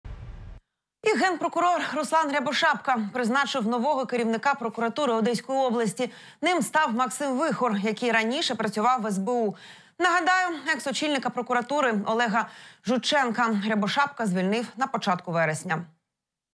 Призначено нового керівника прокуратури Одеської області
Генпрокурор Руслан Рябошапка призначив нового керівника прокуратури Одеської області.

Ним став Максим Вихор, який раніше працював в СБУ.

Нагадаємо, ексочільника прокуратури Олега Жученка Рябошапка звільнив на початку вересня.